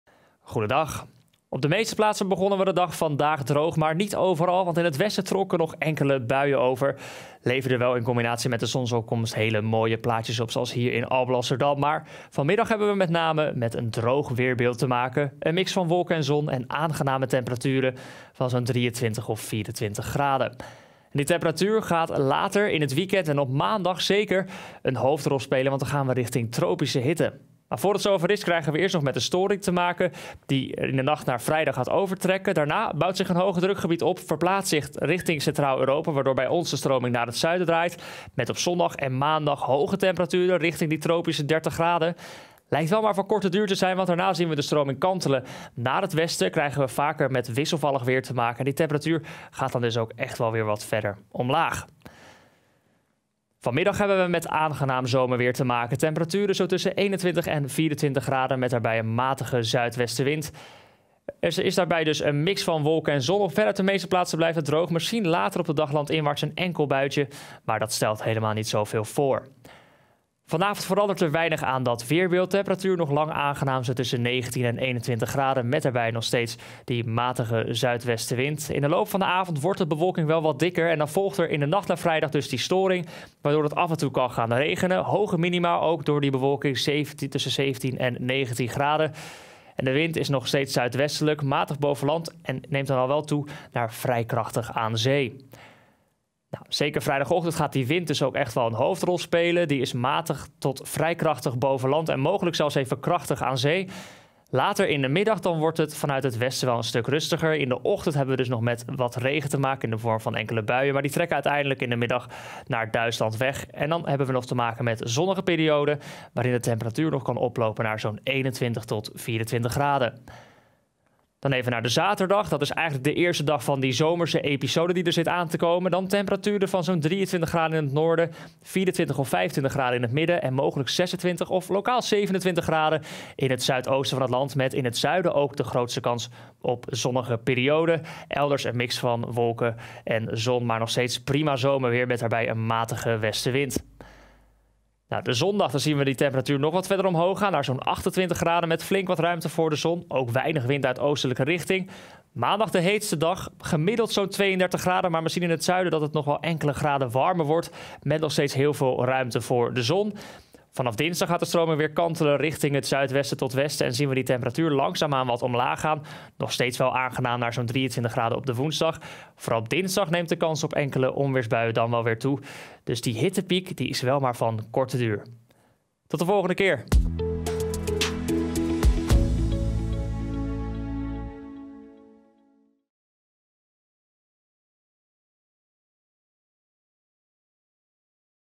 Weerbericht: enkele buien op vrijdag, daarna droger en warmer!
Vandaag is het een aangename zomerdag, maar morgen is het tijdelijk wisselvalliger. Vanaf zaterdag wordt het zomers en richting maandag zelfs tropisch. Donny de Koning vertelt je alle details!

Vandaag is het zomers tot tropisch warm. De komende dagen doet de temperatuur een stapje terug, maar er zijn signalen dat het weer zeer warm gaat worden.